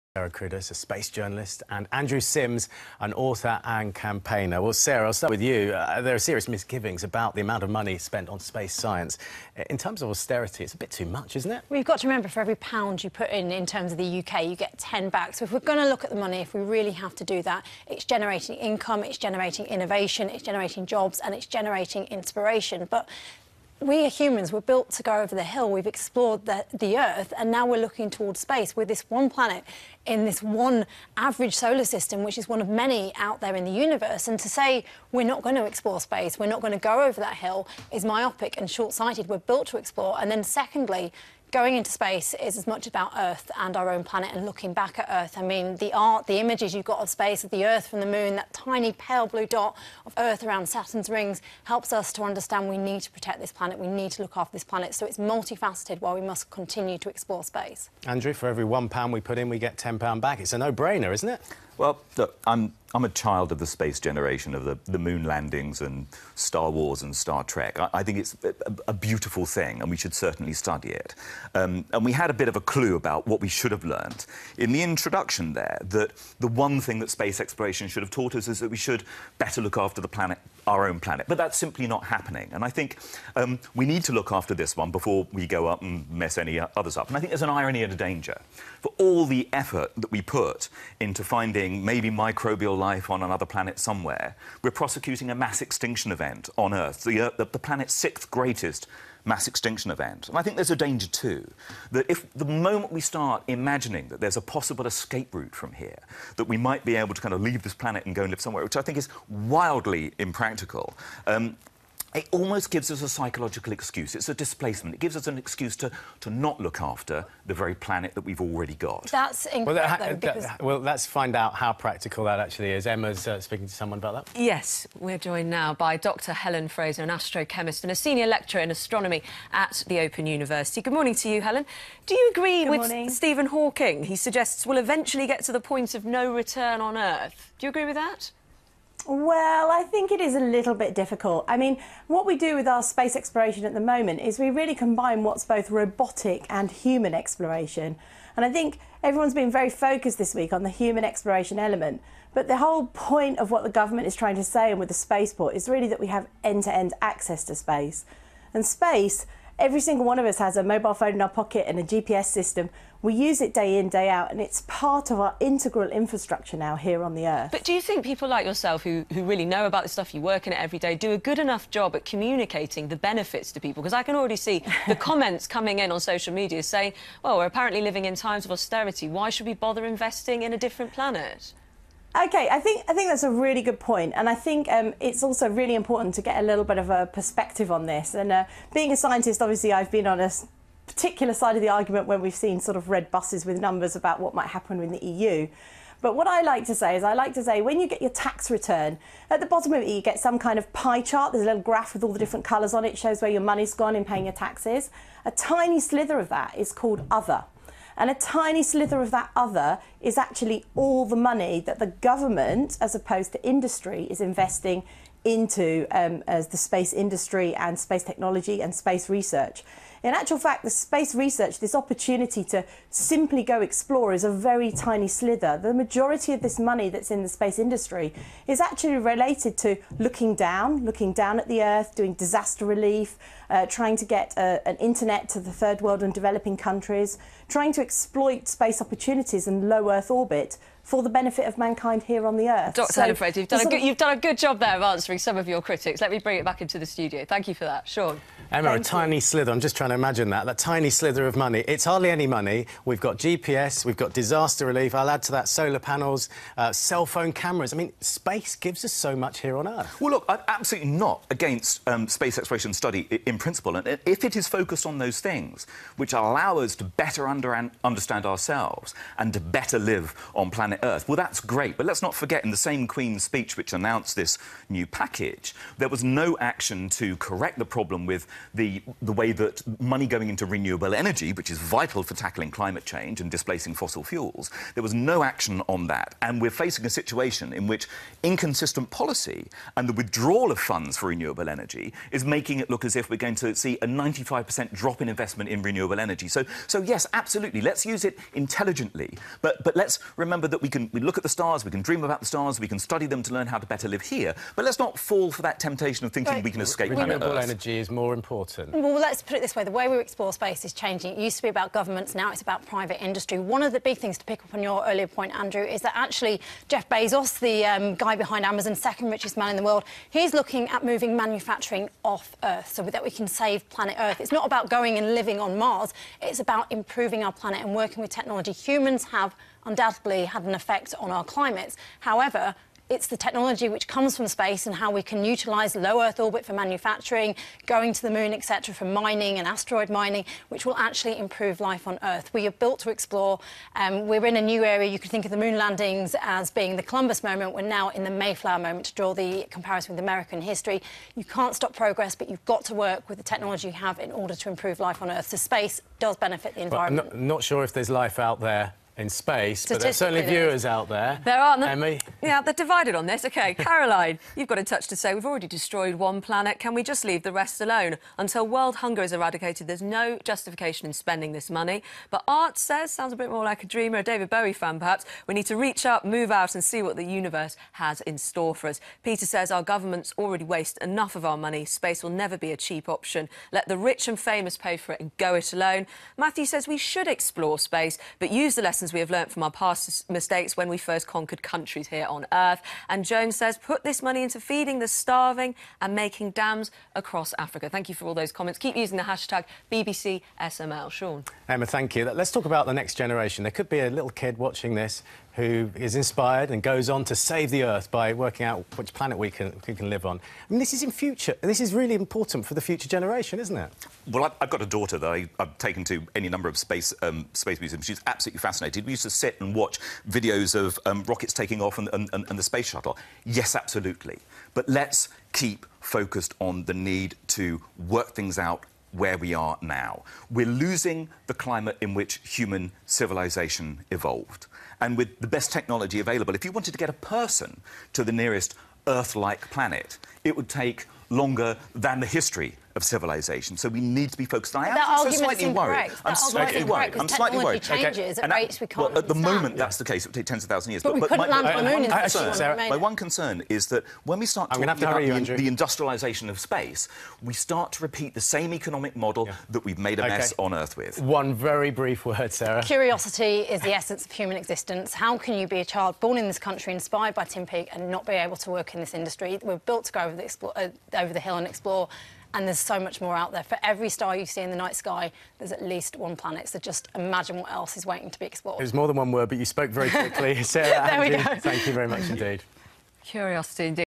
SML Debate: Should we invest in space exploration?
Space journalist and Astrophysicist Sarah Cruddas, Space Scientist Dr Helen Fraser & Andrew Simms, an Author & Campaigner debated if we should invest more in space exploration.